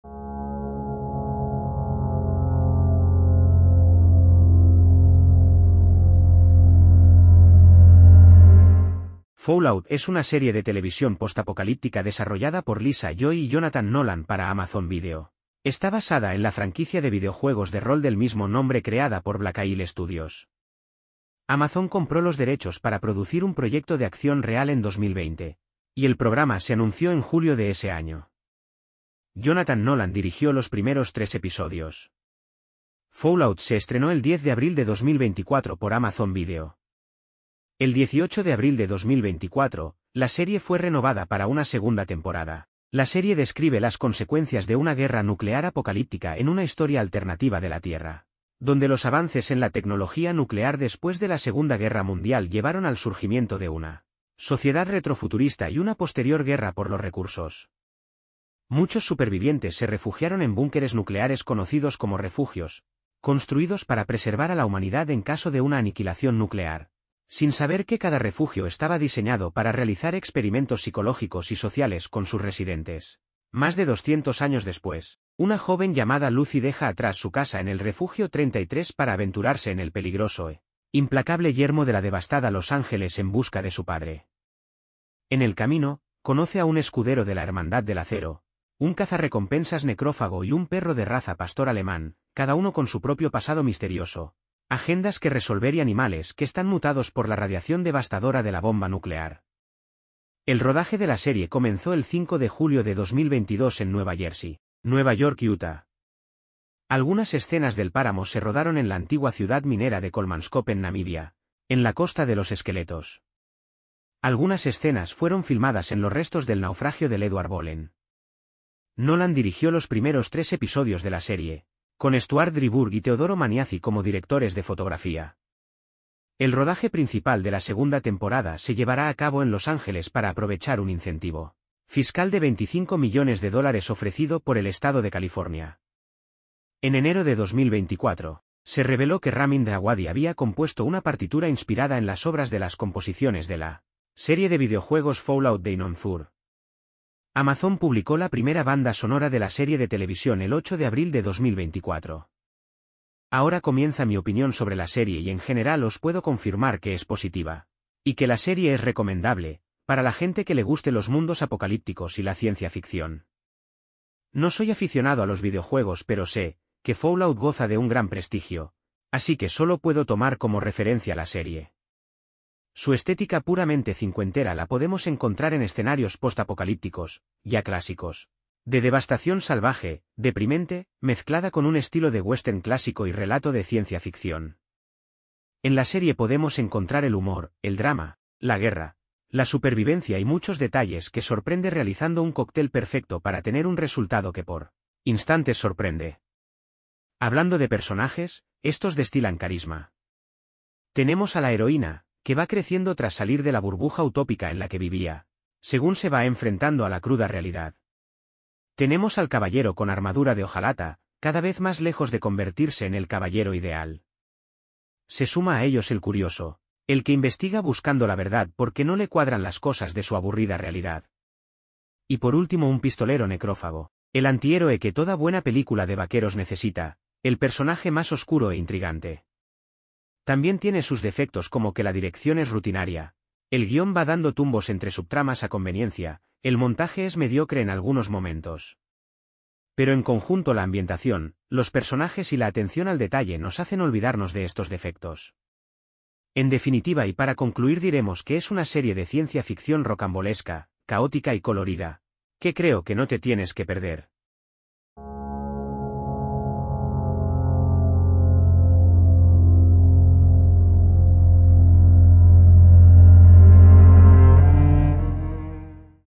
Series : Fallout ( 2024 ) ( Comentarios y Crítica )
Antes de seguir leyendo y si el video te pareció interesante...

Serie de TV (2024). 1 temporada. 8 episodios. Basado en una de las sagas de videojuegos más importantes de todos los tiempos, es la historia de los que tienen y de los que no tienen en un mundo en el que prácticamente no queda nada que tener. 200 años después del apocalipsis, los apacibles habitantes de los refugios de lujo se ven obligados a regresar al infierno irradiado que dejaron sus antepasados y se sorprenden al descubrir que les espera un universo increíblemente complejo, alegremente extraño y muy violento.